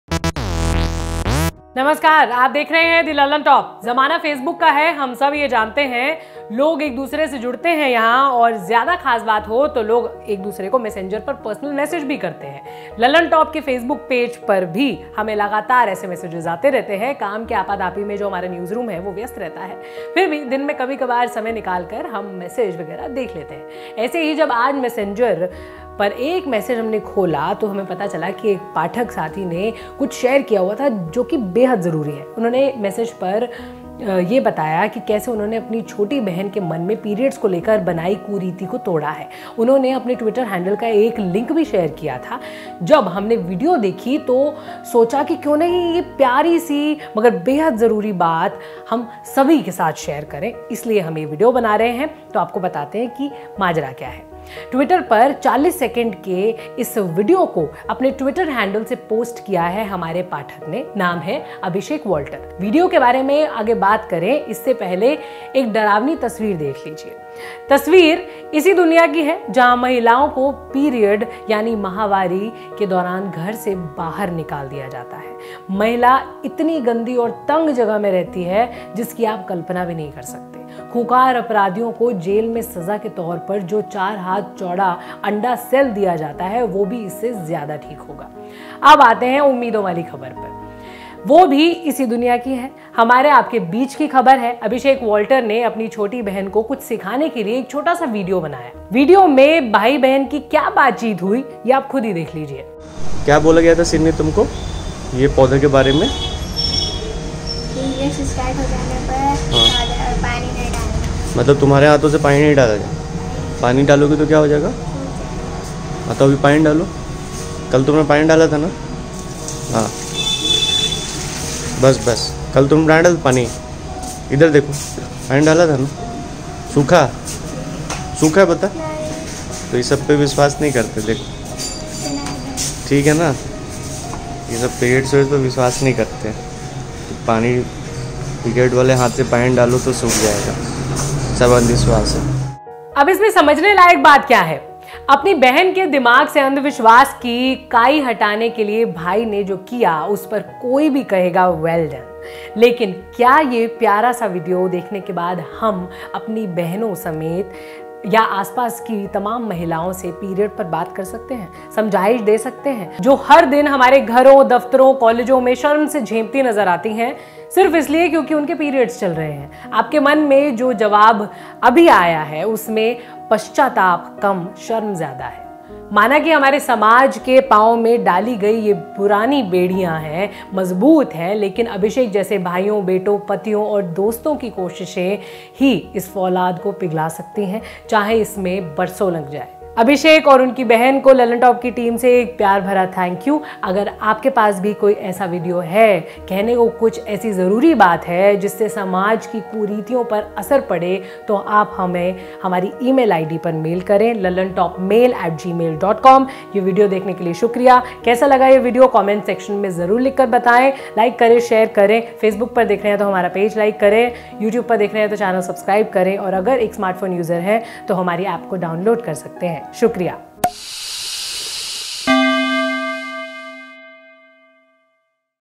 Bihar के Abhishek Walter ने अपनी बहन को Periods पर एक ज़रूरी सीख दी | Superstitions
Misconceptions\ Superstitions about periods: Brother teaches a good and an eye opening lesson to his younger sister who has been made to believe the superstitions about mensuration.